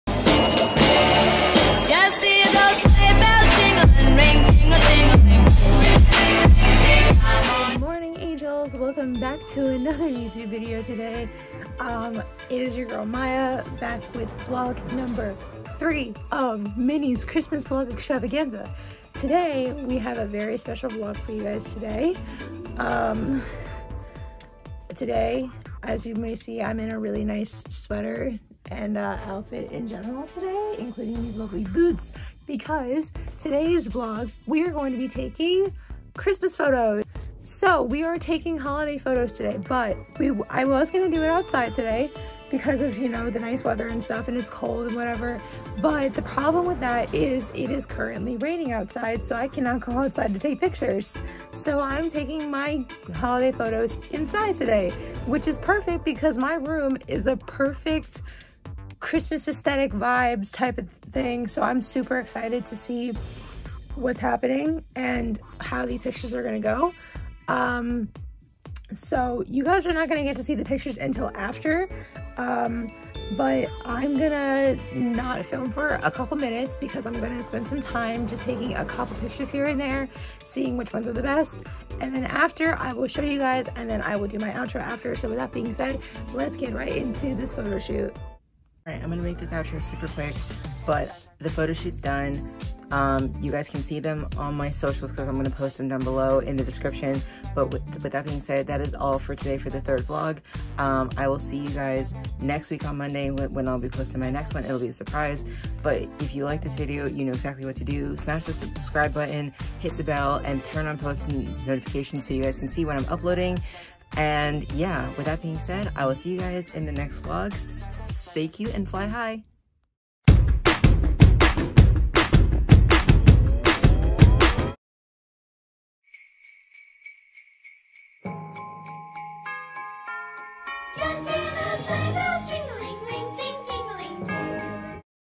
MCVE: Vlog 3🎄: Christmas Photo Shoot❄️😍📸😘
So this is my 3 Vlog for Mya’s Christmas Vlog Extravaganza! Today’s video was a photo shoot! If y’all wanna see the results on it, the pictures are on my insta and Snapchat. Follow me on both as well as all my other socials!